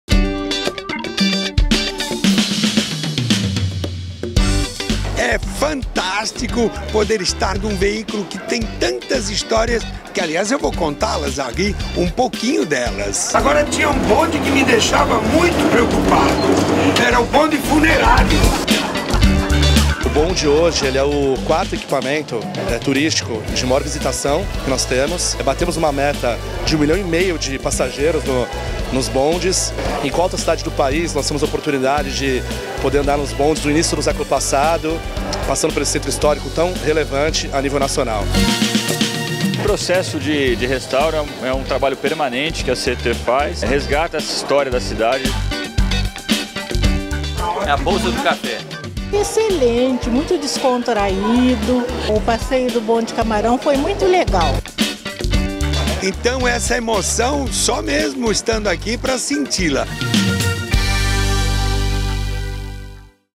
Revitalizado, Bonde Camarão volta a circular
O cinquentão recauchutado está novamente circulando pelo Centro Histórico. O carro prefixo 40 voltou à ativa nesta terça (11), após ampla reforma. No final da manhã, ele fez duas viagens temáticas para lembrar que há 108 anos o bonde elétrico começava a rodar em Santos. Saiba mais